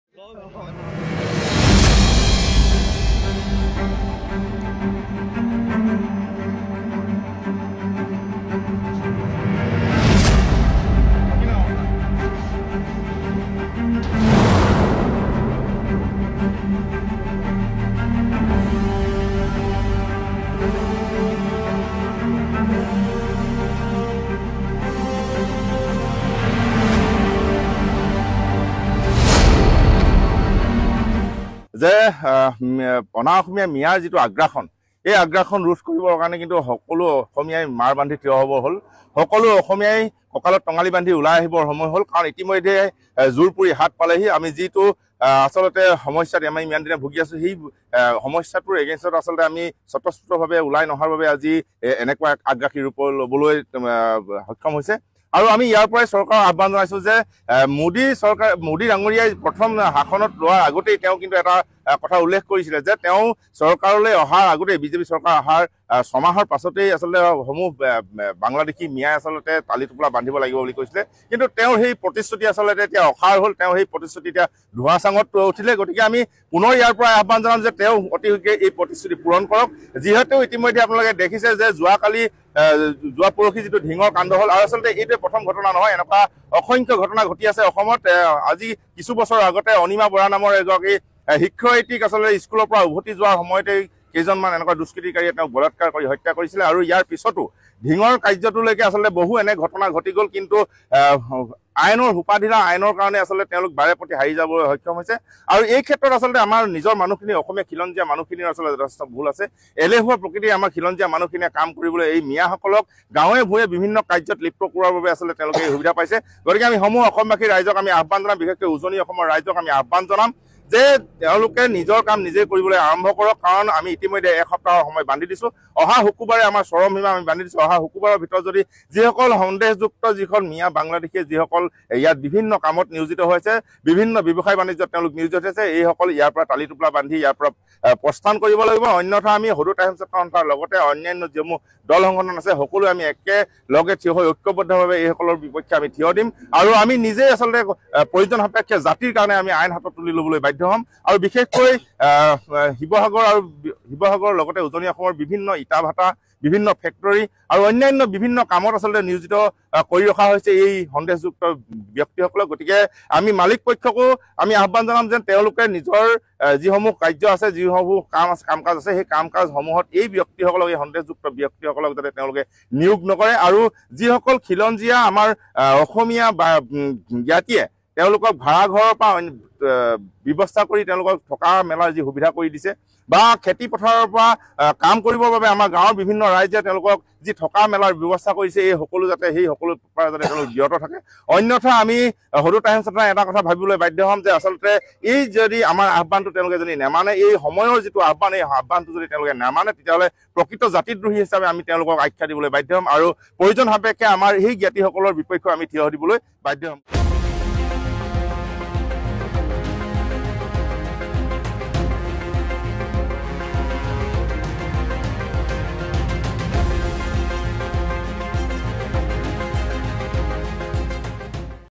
শিৱসাগৰৰ বিভিন্ন প্ৰান্তত দল সংগঠনে আৰম্ভ কৰিছে মিঞাৰ বিৰুদ্ধে অভিযান
▶️ আজিৰে পৰা আটাছুৰ নেতৃত্বত আৰম্ভ হৈছে অভিযান
▶️ শুক্ৰবাৰৰ ভিতৰত সন্দেহযুক্ত মিঞাক শিৱসাগৰ এৰিবলৈ সকীয়নি আটাছুৰ